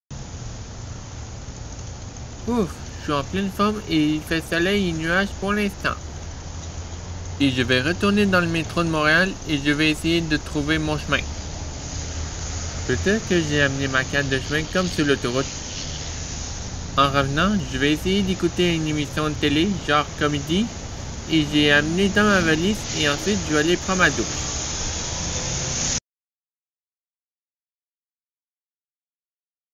Montréal Le Film - Partie 11
Je suis allé me promené dans le centre ville et je vais retourné dans le métro montréal :)